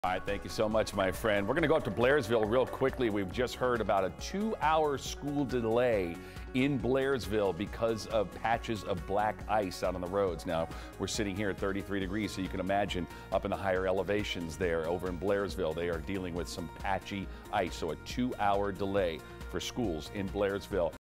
Two hour school day in Blairsville
The delay is due to concerns of small patches of black ice around the county.